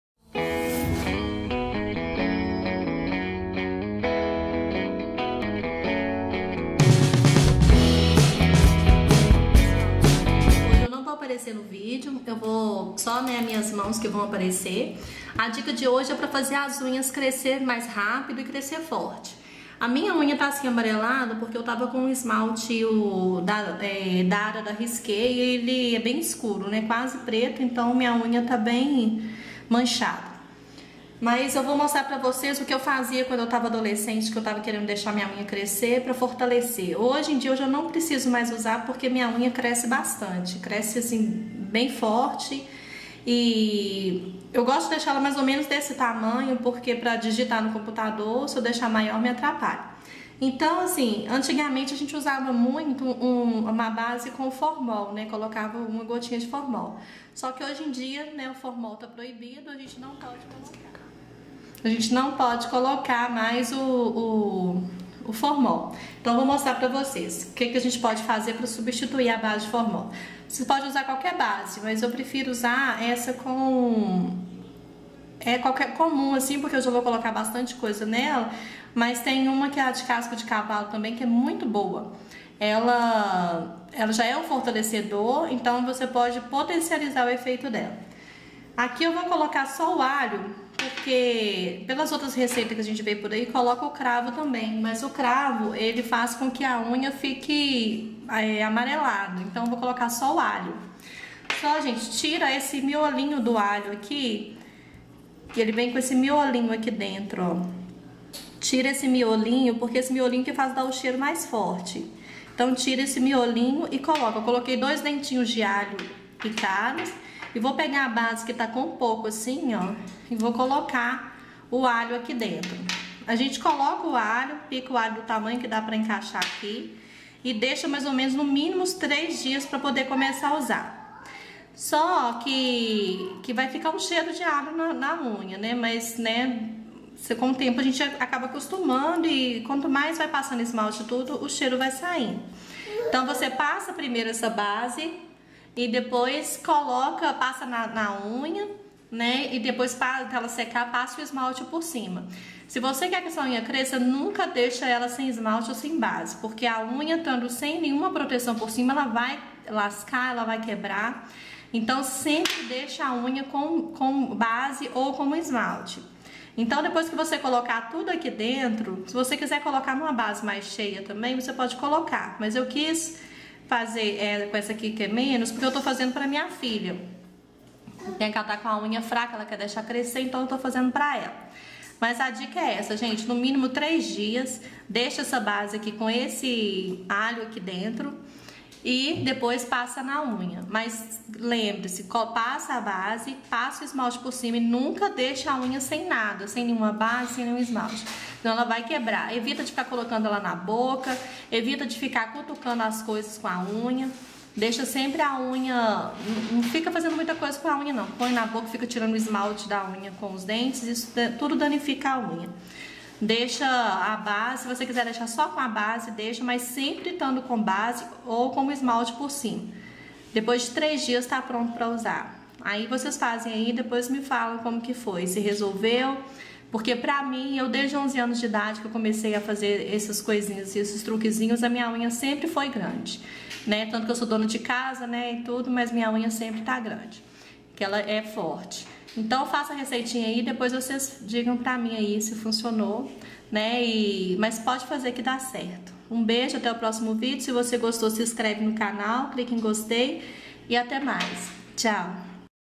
Como ter unhas Grandes e Fortes
Essa receitinha é do tempo da vovó e junto com as dicas que eu dou no vídeo suas unhas vão ficar enormes e duras ,espero que gostem